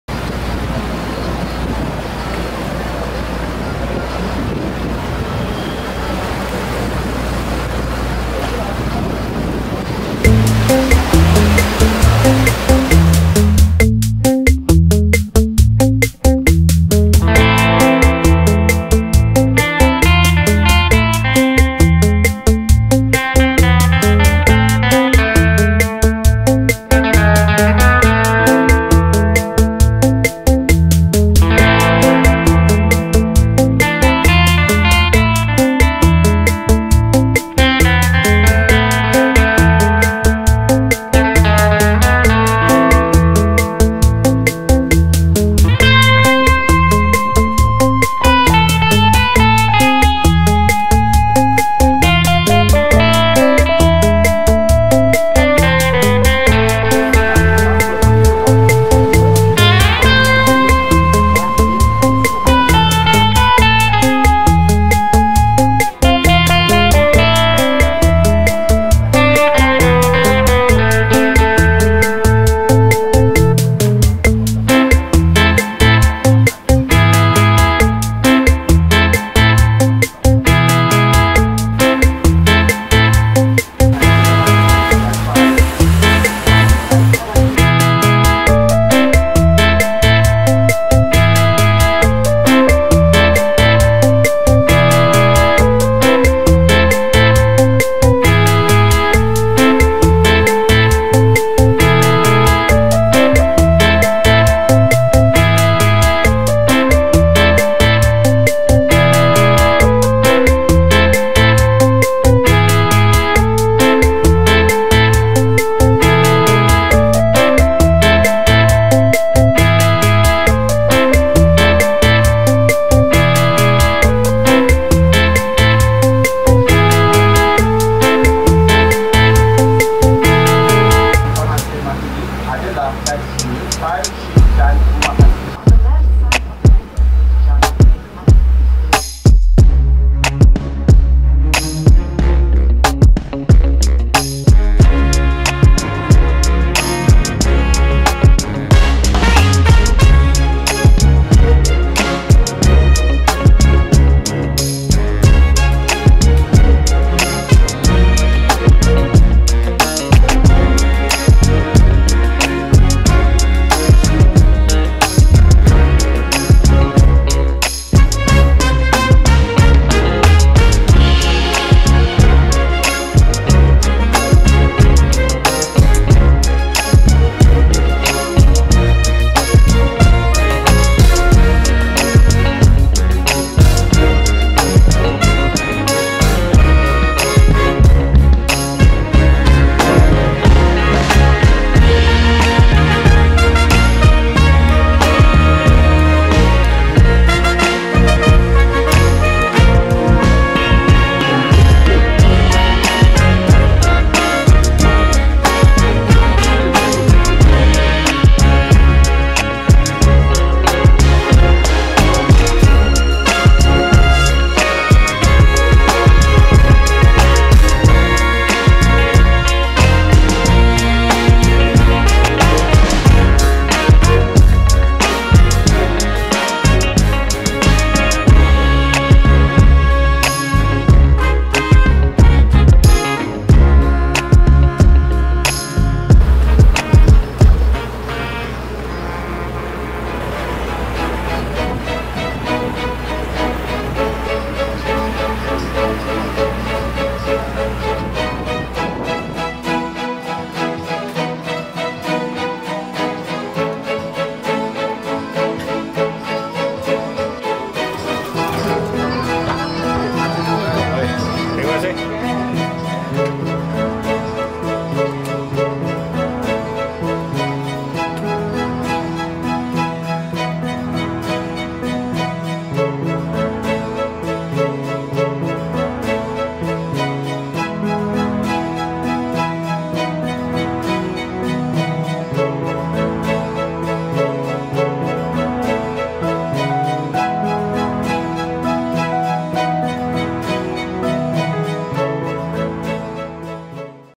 Melaka River Cruise 在馬六甲河坐船
剛剛和家人去馬六甲走走，嘗試了一趟40分鐘的游船河，感覺比想象的好，因爲沿途上看到的建築都被美化了，感覺很有誠意，也讓外國游客留下好印象。對上一次和爸爸游船河是幾年前在廣州珠江，現在還可以和爸爸一起出來走走真的是感恩。